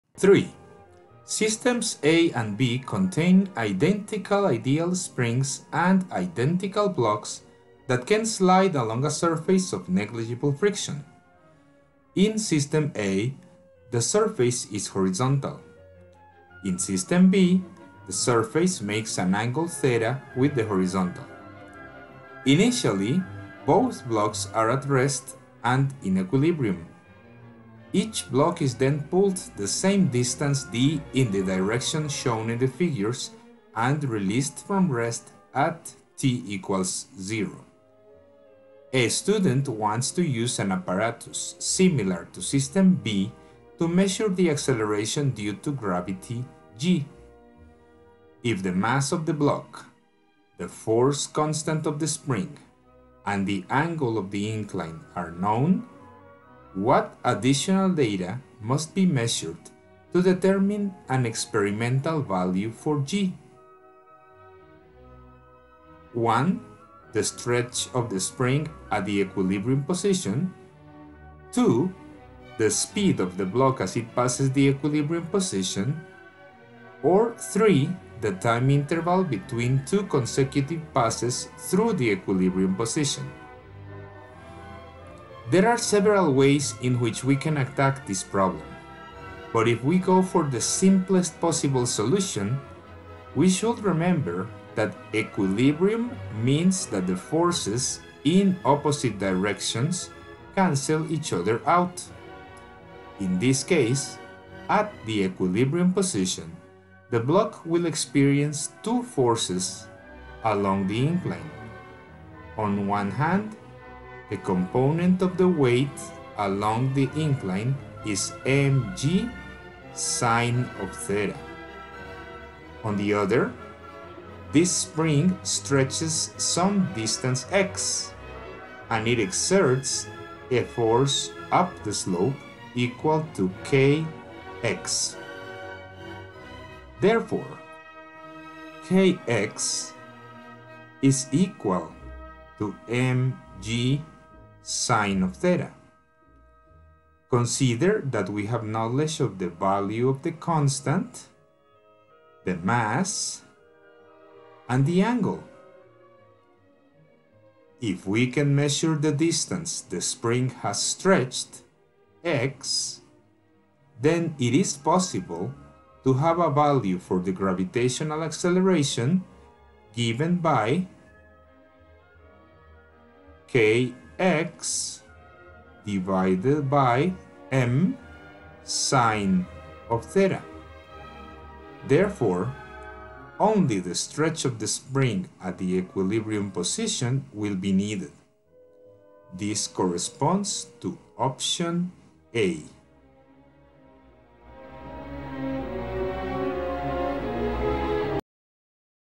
3.2-02 Using a Spring to Determine the Value of the Acceleration due to Gravity
This video analyzes what should be the minimum data needed to calculate an experimental value of the gravitational acceleration using the laws of motion on a block hanging from a spring that obeys Hooke's law.